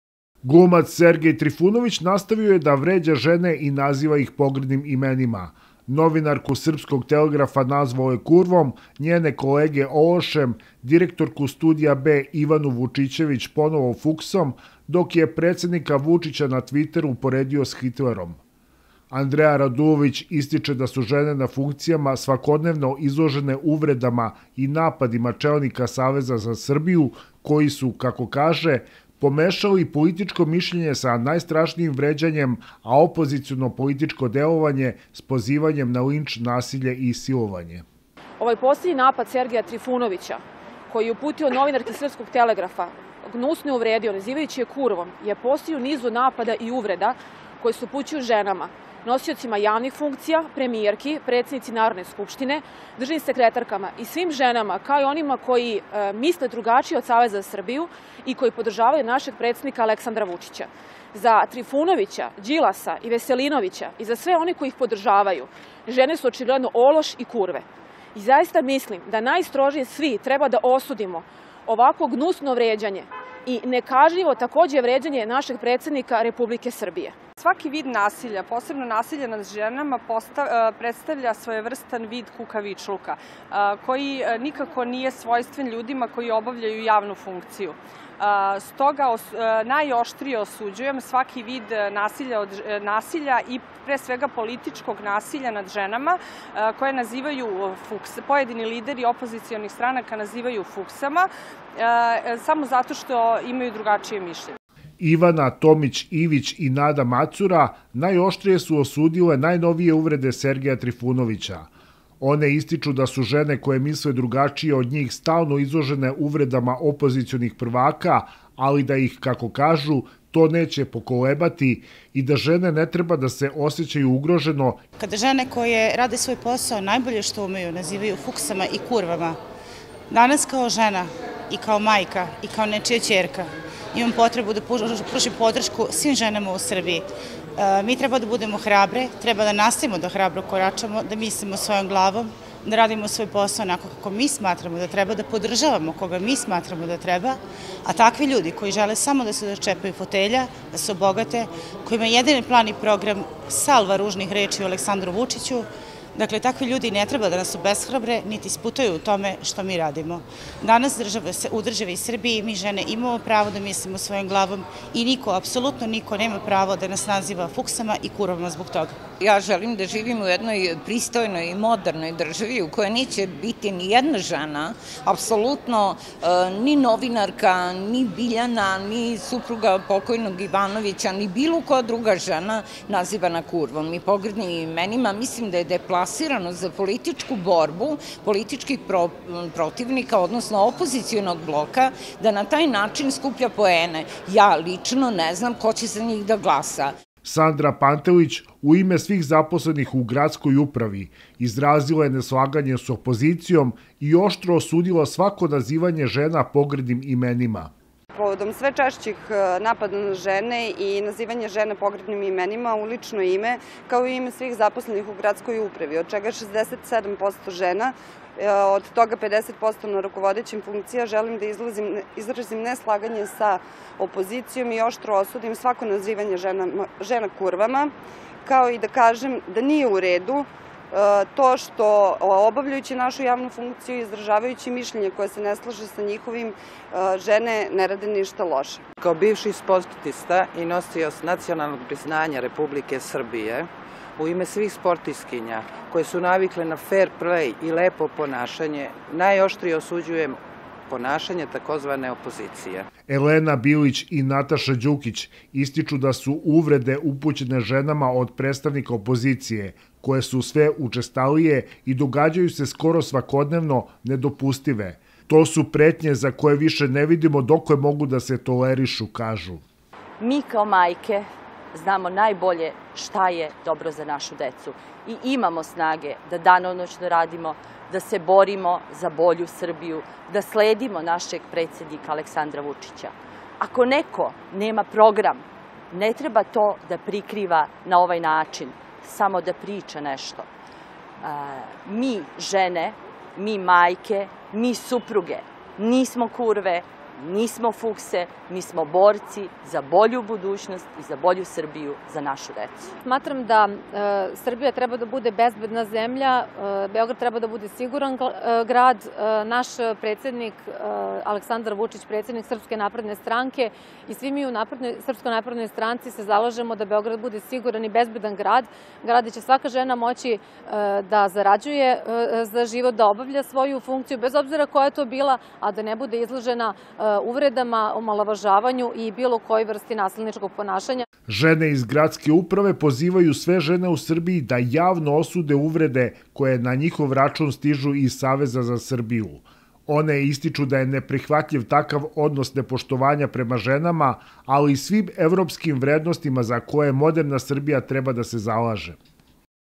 SNS: Vređanje žena je nedopustivo
Predstavnice Srpske napredne stranke koje obavljaju javnu funkciju u Upravi grada Beograda najoštrije su osudile uvrede koje na račun žena iznose predstavnici opozicije i istakle da je vređanje žena nedopustivo i da ne može više da se toleriše.